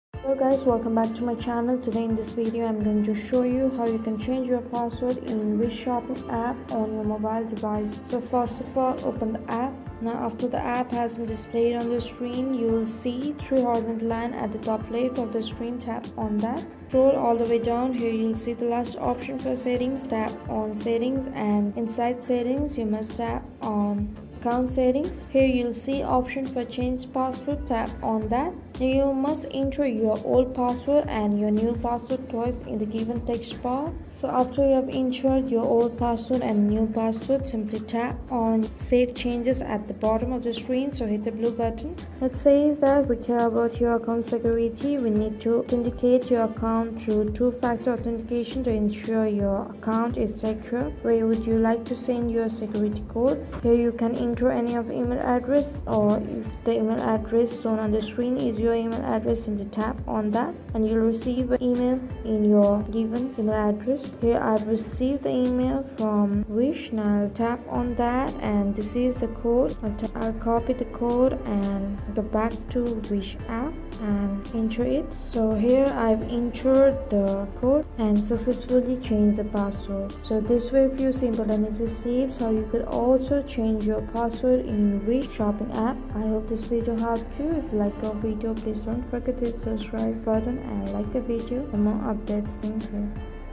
How To Change Password in Wish Account 2022?
Change Wish Account Password on Mobile:

This video will guide you to change your password on your Wish account.
1) Open your Wish App and make sure you are already logged into your account.
2) Now once your profile opens up on the top right corner of the screen you will see "Three Horizontal Lines"
3) Then tap on " Settings" option.
4) Then tap on "Account Settings" option.
5) Then tap on "Change Password" option. Now enter your new password, your old password and you need to retype your new password as well.
6) Now tap on "Save Changes" option.
7)Now follow the instructions till the end, and that's how you can change password in Wish App.

0:08 Change Wish Account Password
1:26 Outro